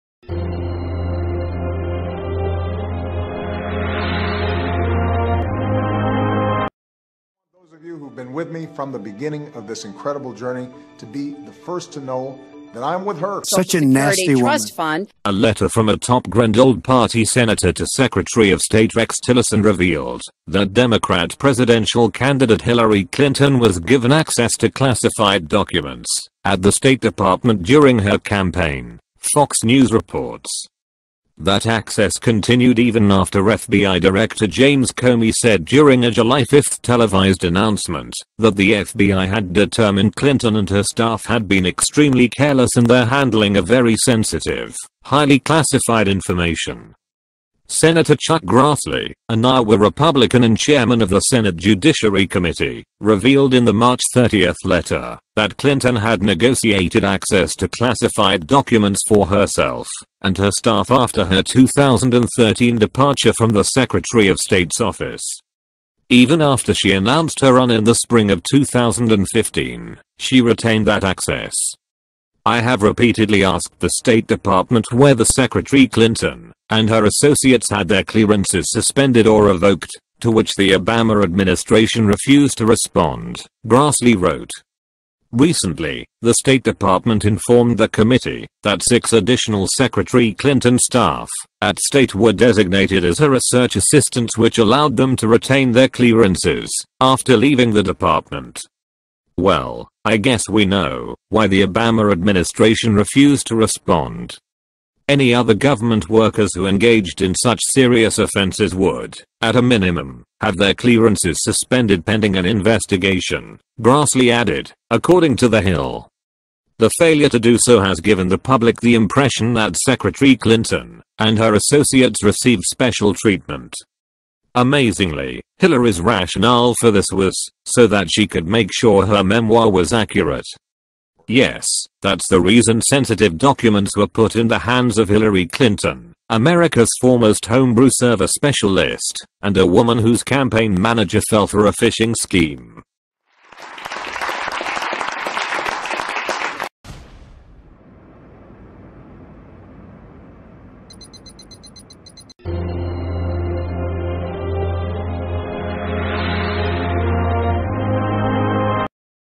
Sen. Chuck Grassley: Obama Gave Hillary Classified State Dept. Access Even After She Left!
Obama gave Hillary classified State Dept. access even after she left. A letter from a top GOP senator to Secretary of State Rex Tillerson revealed, that Democrat presidential candidate Hillary Clinton was given access to classified documents, at the State Department during her campaign, Fox News reports. That access continued even after FBI Director James Comey said during a July 5 televised announcement, that the FBI had determined Clinton and her staff had been “extremely careless in their handling of very sensitive, highly classified information.”

Sen. Chuck Grassley, an Iowa Republican and chairman of the Senate Judiciary Committee, revealed in the March 30 letter that Clinton had negotiated access to classified documents for herself, and her staff after her 2013 departure from the secretary of state’s office. Even after she announced her run in the spring of 2015, she retained that access. “I have repeatedly asked the State Department whether Secretary Clinton, and her associates had their clearances suspended or revoked, to which the Obama administration refused to respond,” Grassley wrote. “Recently, the State Department informed the committee that six additional Secretary Clinton staff, at State were designated as her research assistants which allowed them to retain their clearances, after leaving the Department.” Well, I guess we know why the Obama administration refused to respond.

“Any other government workers who engaged in such serious offenses would, at a minimum, have their clearances suspended pending an investigation,” Grassley added, according to The Hill. “The failure to do so has given the public the impression that Secretary Clinton, and her associates received special treatment.” Amazingly, Hillary’s rationale for this was so that she could make sure her memoir was accurate. Yes, that’s the reason sensitive documents were put in the hands of Hillary Clinton, America’s foremost homebrew server specialist, and a woman whose campaign manager fell for a phishing scheme.